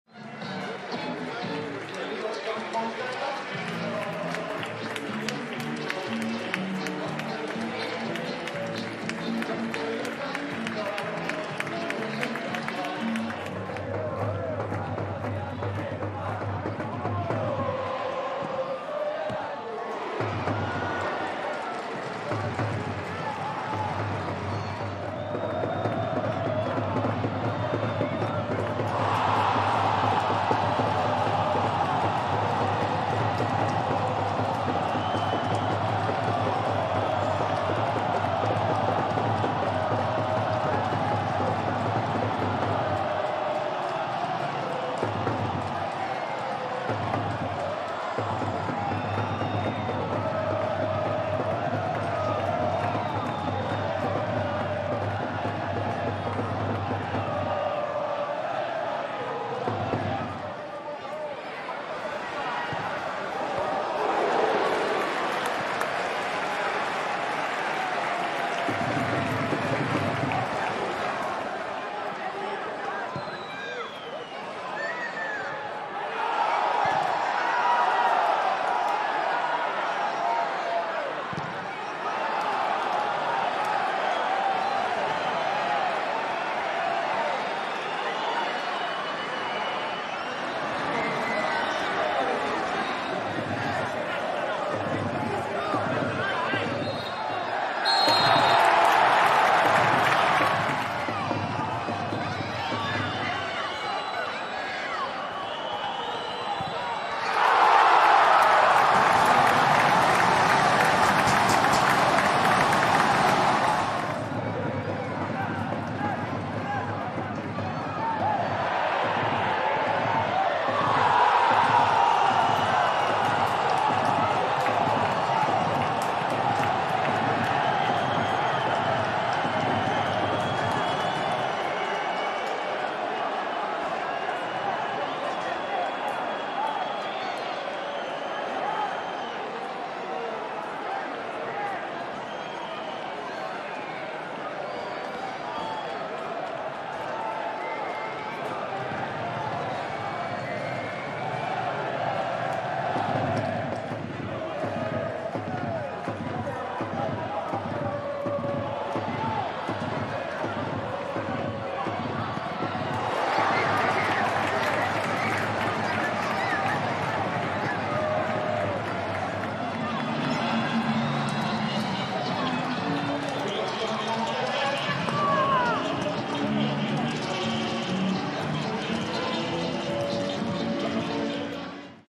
Resumen: ADA 1-2 ABP
Los mejores momentos del triunfo en Santo Domingo